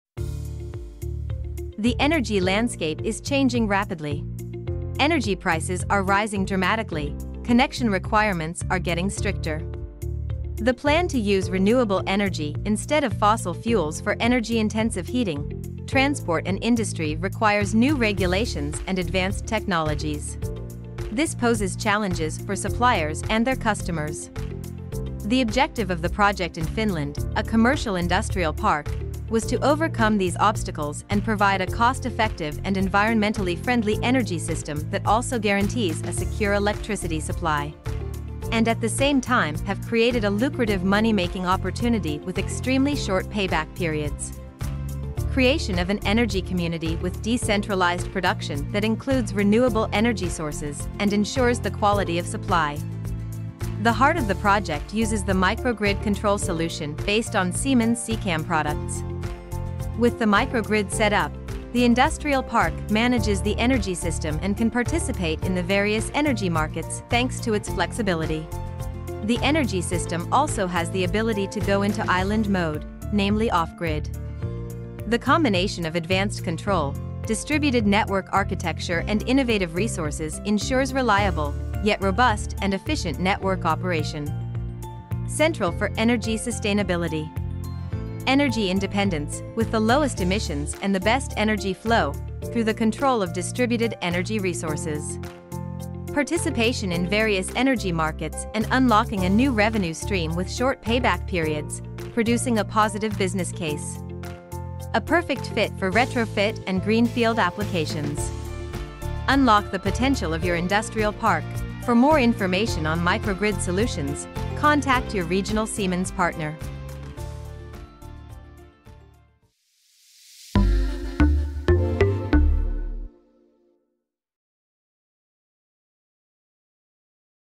Unlocking the Potential of your Industrial Park with Microgrid Solutions from Siemens
In the rapidly changing energy landscape, rising energy prices and stricter connection requirements are posing challenges for suppliers and customers. But there's a solution. In Finland, a commercial industrial park successfully overcame these obstacles by implementing a cost-effective and environmentally friendly energy system. By creating an energy community with decentralized production and utilizing Siemens' Microgrid Control solution, they achieved energy independence, participated in energy markets, and unlocked new revenue streams with short payback periods. Learn how microgrid solutions can transform your industrial park. Contact your regional Siemens partner today.